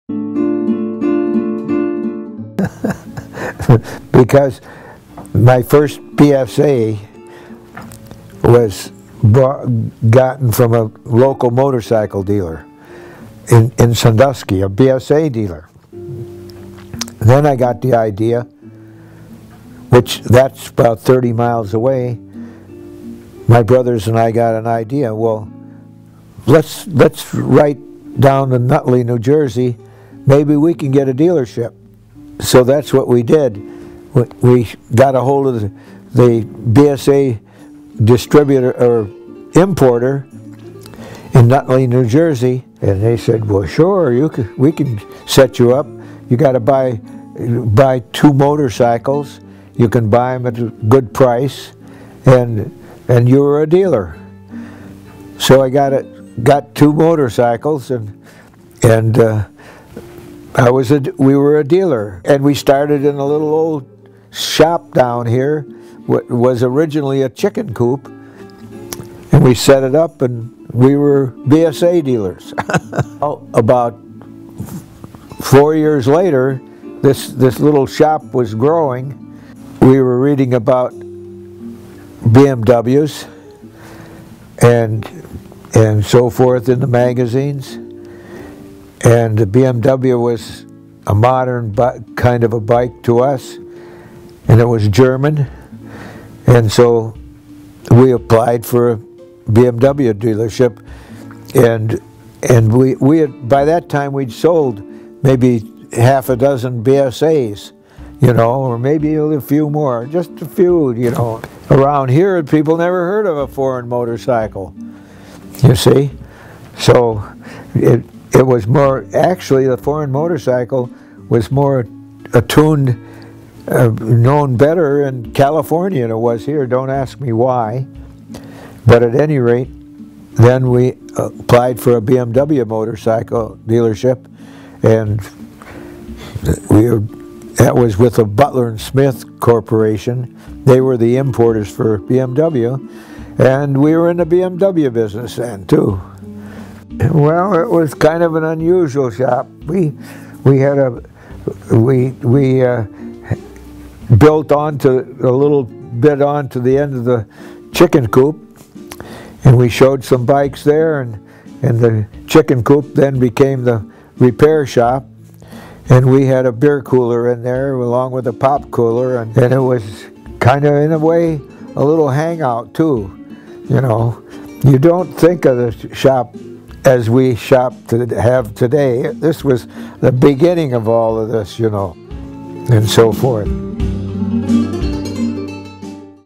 John Penton: Changing the Game (part III)
AMA Motorcycle Hall of Famer John Penton talks about how he and his brothers became a BSA and BMW dealer.